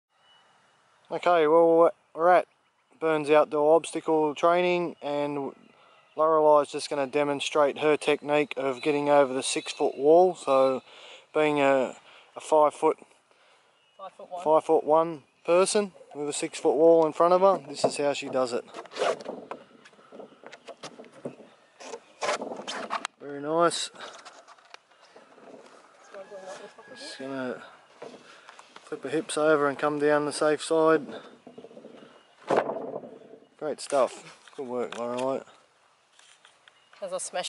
5'1ft vs 6ft wall climb obstacle
Demonstration on a wall climb obstacle. One of many techniques.